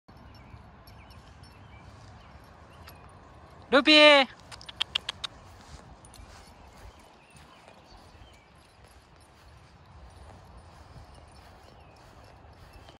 [SADK34] iPhone 11 Pro - 4K Video Recording Test - Schnauzer Running 슈나우저 달리기 고화질 녹화
Recording of Ruby's run in field
Recorded from iPhone 11 Pro